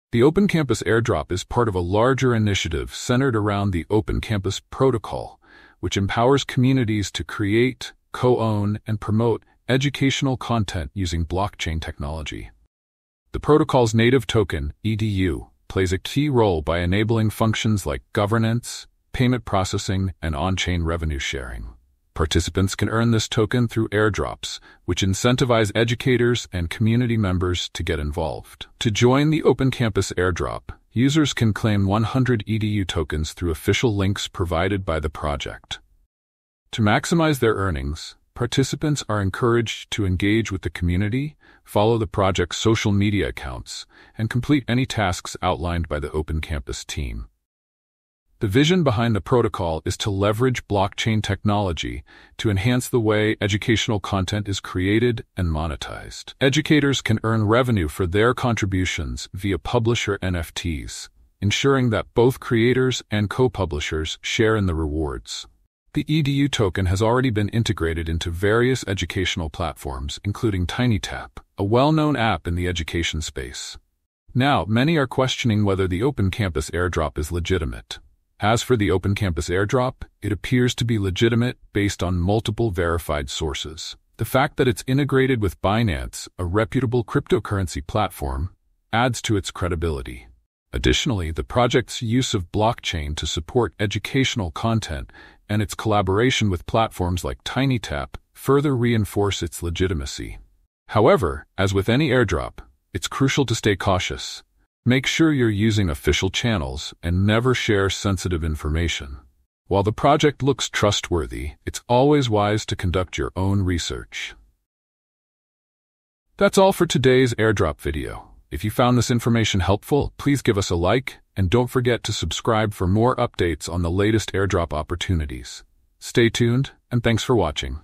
Open Campus Airdrop Review - Legit or Scam?
The Open Campus airdrop is part of a larger initiative centered around the Open Campus protocol, which empowers communities to create, co-own, and promote educational content using blockchain technology.

The protocol's native token, EDU, plays a key role by enabling functions like governance, payment processing, and on-chain revenue sharing.

Participants can earn this token through airdrops, incentivizing educators and community members to get involved.

To join the Open Campus airdrop, users can claim 100 EDU tokens through official links provided by the project.